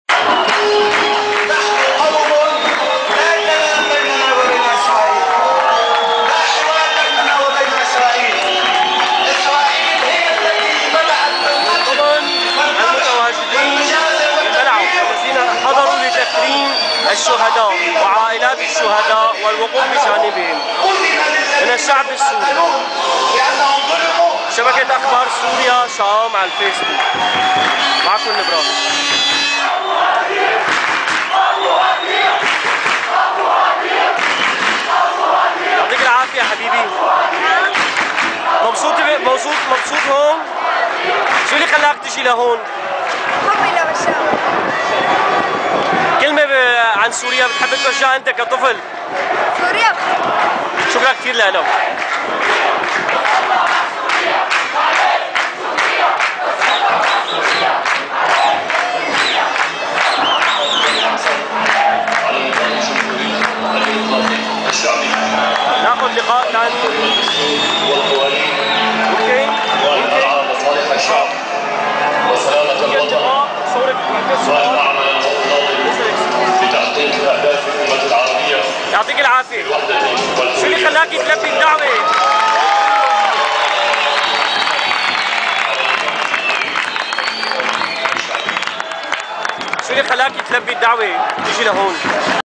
التكريم الشعبي لذوي الشهداء في سوريا /6/
من ضمن الحضور كان لنا لقاءا وجولة بين أجواء الحفل عالمدرجات
كونوا معنا عالفيس بوك ضمن صفحة شآم